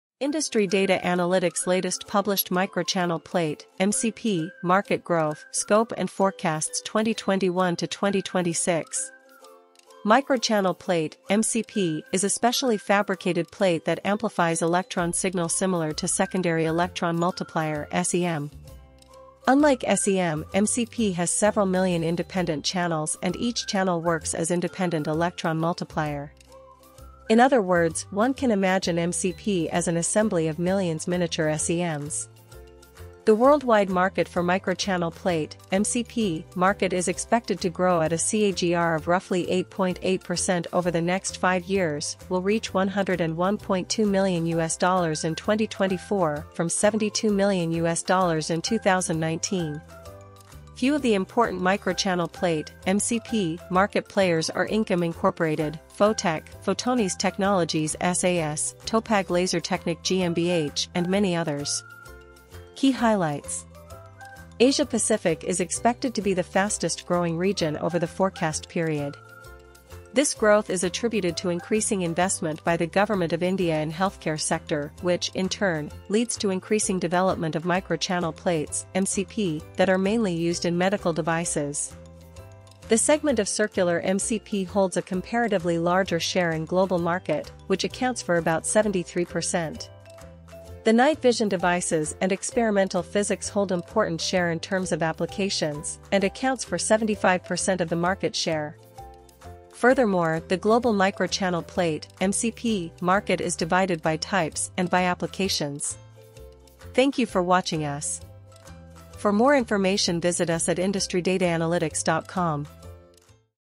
Micro-channel Plate (MCP) Market 2021 | Industry Data Analytics | IDA
Industry Data Analytics latest Publish Micro-channel Plate (MCP) Market Research Report 2021 - Growth, Trends And Forecast to 2026 By Types, By Application, By Regions and By Key Players

Some of the Top companies referred in the market research report includes Hamamatsu Photonics, PHOTONIS, Incom, Baspik, North Night Vision, Tectra GmbH, Topag and others.

The Global Micro-channel Plate (MCP) market size will register a 9.5% CAGR in terms of revenue to reach $ 108.6 million by 2026.